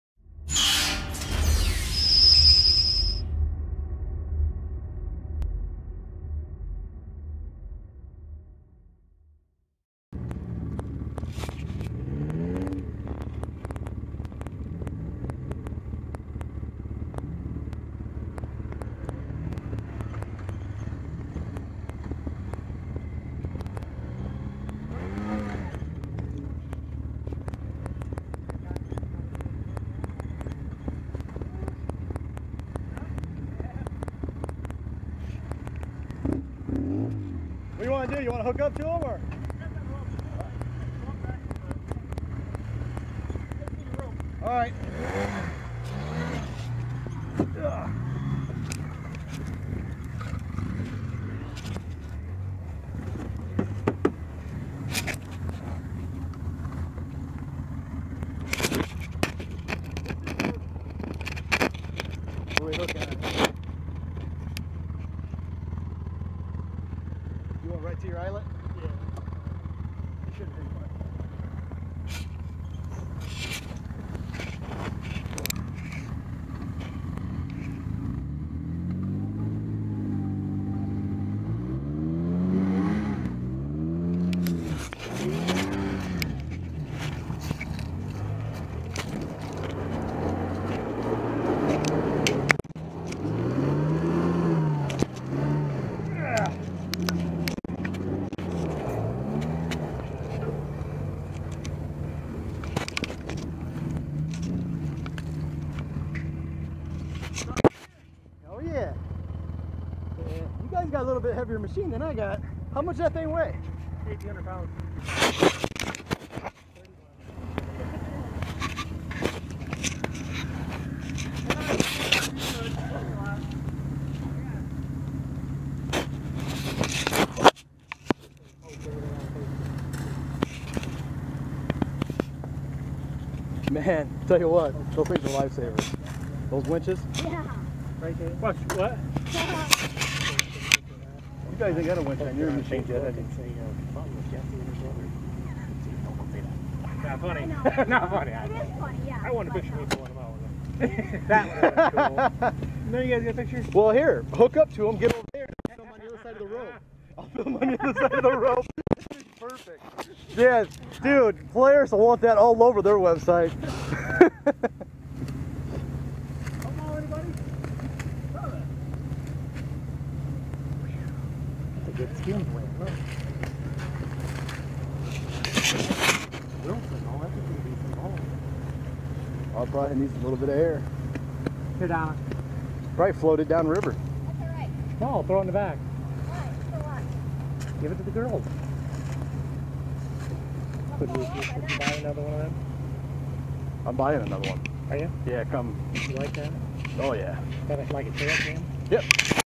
wellsville trails 2013
freinds and I doing some side x side crawin